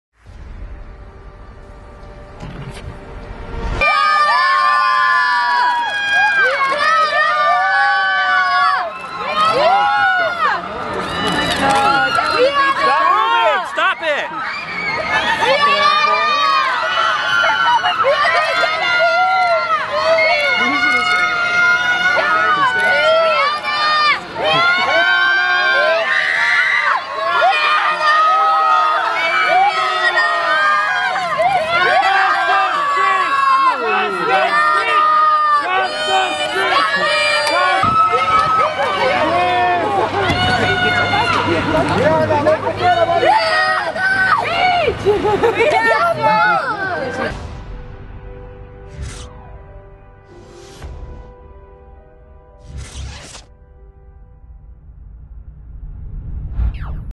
Rihanna arrives to screaming fans at Battleship LA film premiere
Singer Rihanna arrives to screaming fans at Battleship LA film premiere at Nokia Theatre L.A. Live on May 10, 2012 in Los Angeles, California, United States.